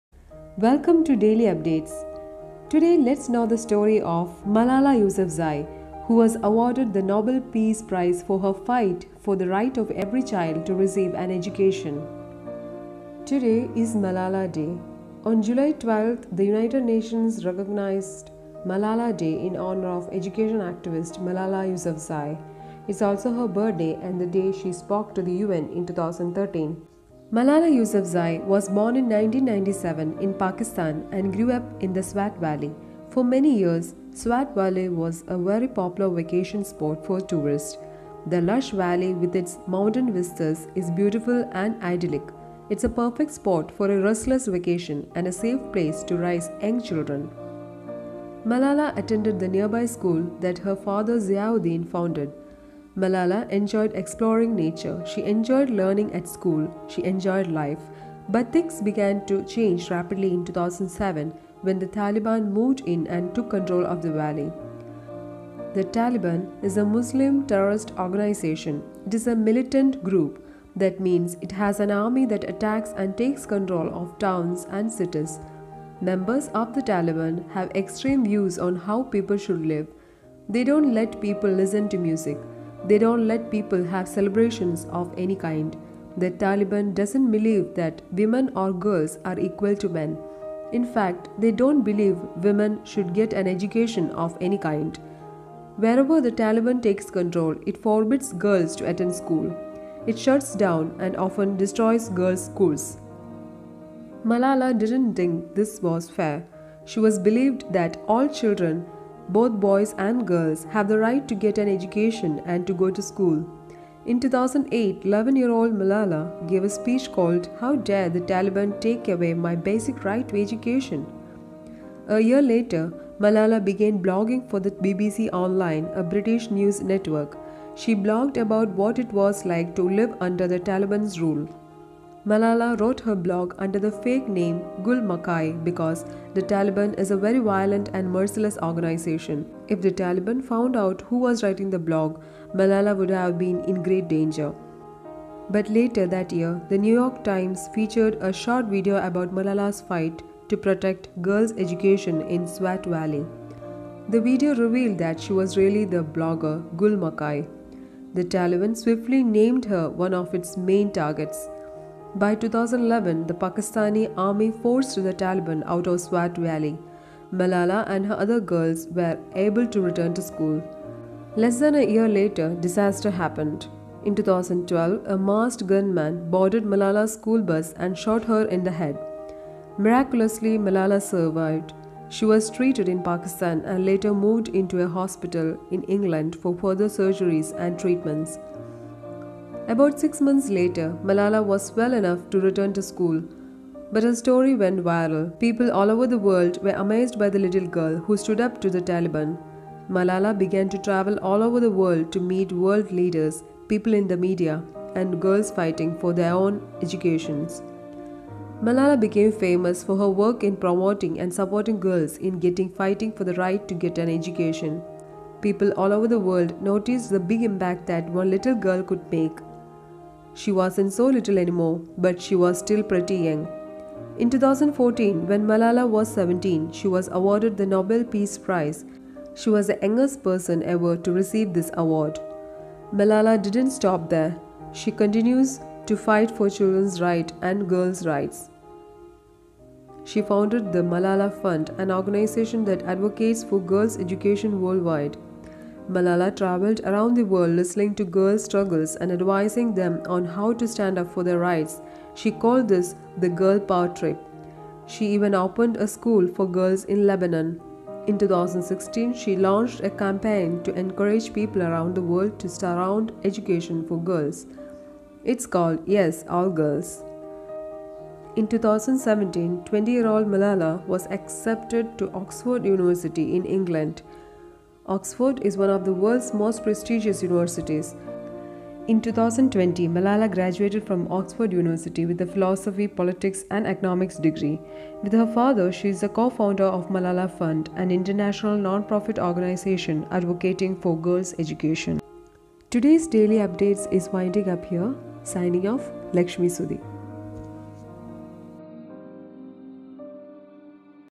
July 12th Malala Day / Story of Malala Youzafsai/Youngest Nobel peace prize Laureate/by LekshmiSudhi
Malala Yousafzai, often referred to mononymously as Malala, is a Pakistani activist for female education and the 2014 Nobel Peace Prize laureate. Awarded when she was 17, she is the world's youngest Nobel Prize laureate, and the second Pakistani and the first Pashtun to ever receive a Nobel Prize.